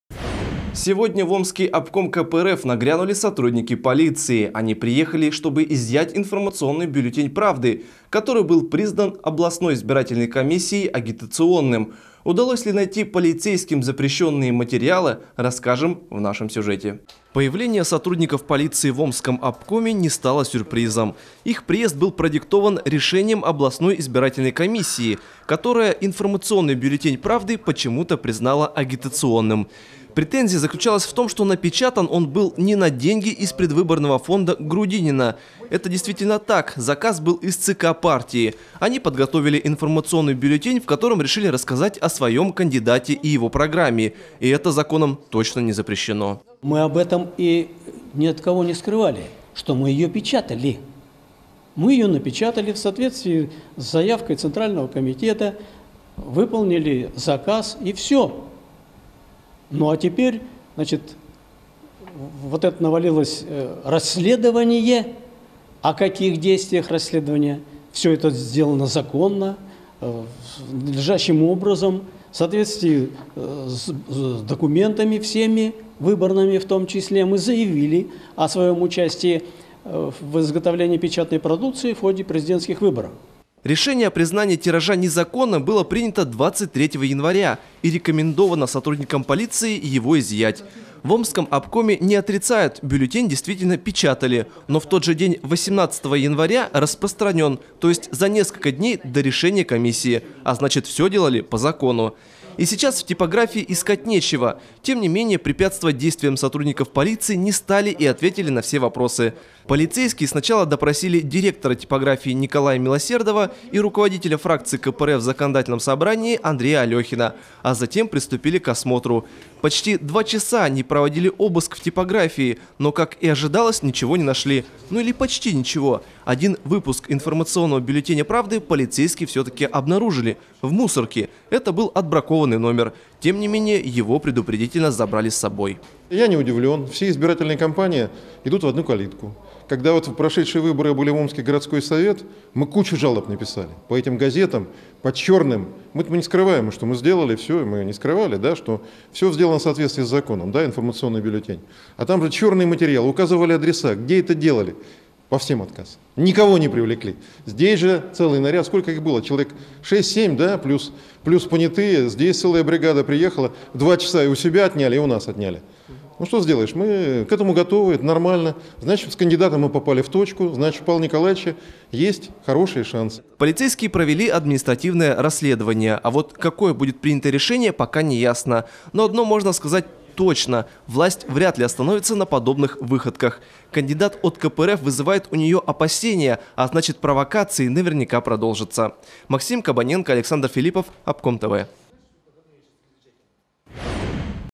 Хроника дня. Искали, но не нашли. 25.01.2018
Сегодня в Омский Обком КПРФ нагрянули сотрудники полиции. Они приехали, чтобы изъять информационный бюллетень "Правды", который был признан областной избирательной комиссией агитационным.